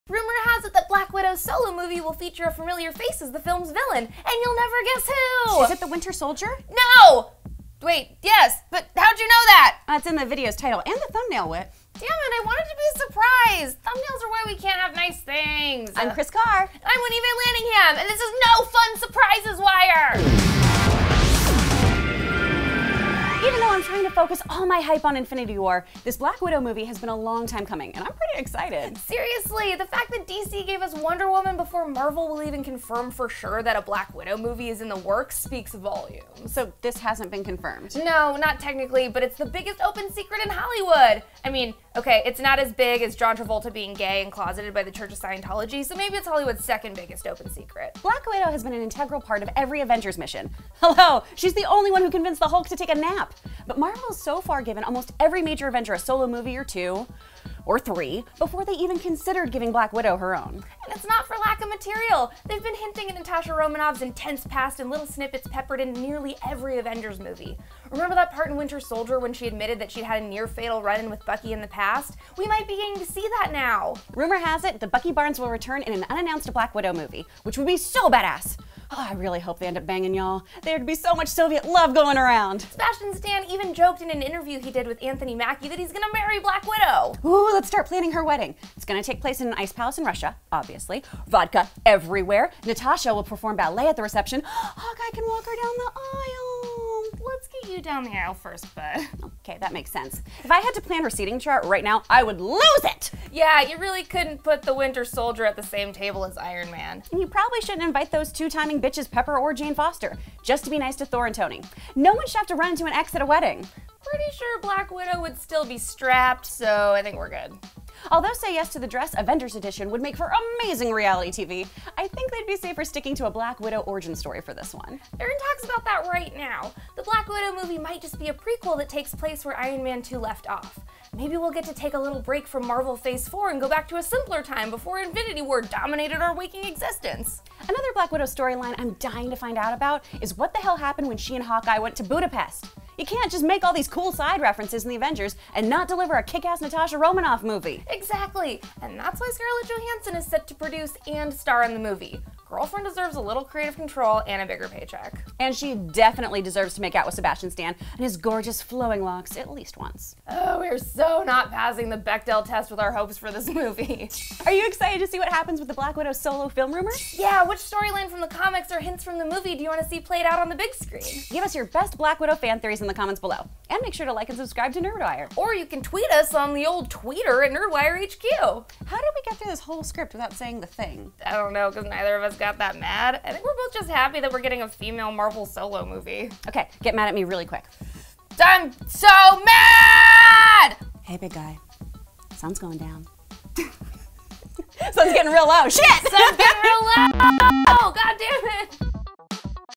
Winter Solider BATTLES Black Widow In New Prequel | NW News
Rumor has it that Black Widow’s solo prequel movie will feature a familiar face as the film’s villain The Winter Soldier! Are you as excited about this as we are?